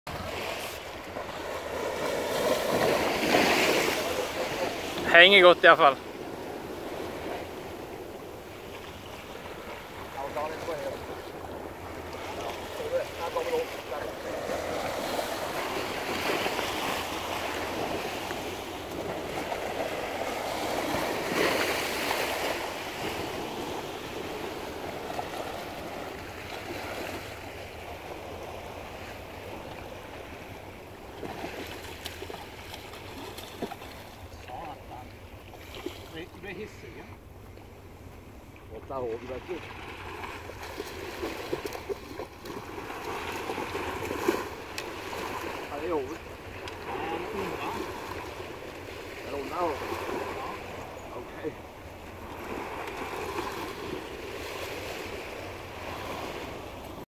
Laks på Beiningen 2017
Pappa får ein på 1.3 kilo . Veldig bra dette :)